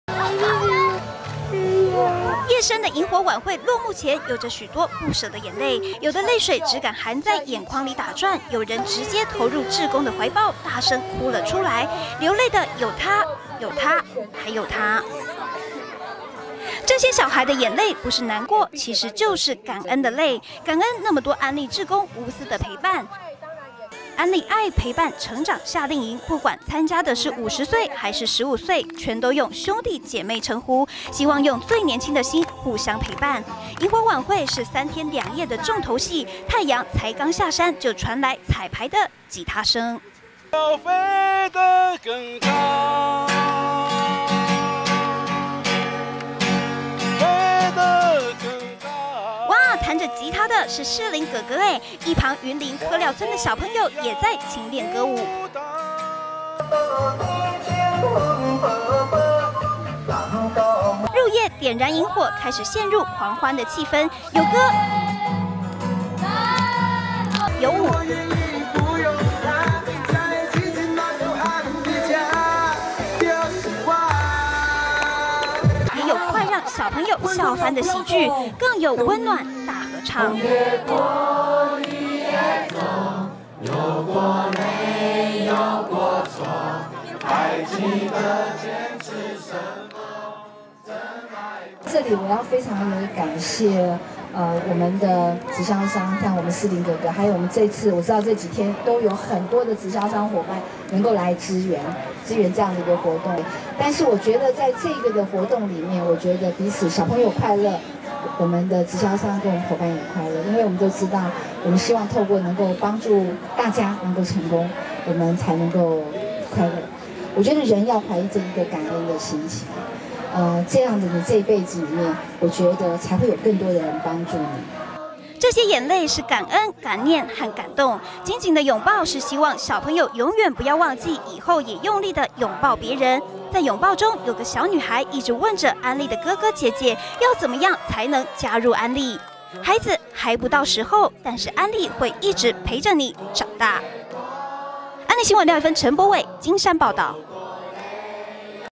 夏令營最終場 溫馨中教導感恩
安麗希望工場慈善夏令營，繼南部和中部之後，這個星期在台北舉行了。安麗希望工場慈善基金會陳惠雯董事長，還特別到金山活動中心，和小朋友們一起同樂，在營火晚會的溫暖氣氛中，她要小朋友在志工的陪伴照顧中，學會兩個字，「感恩」。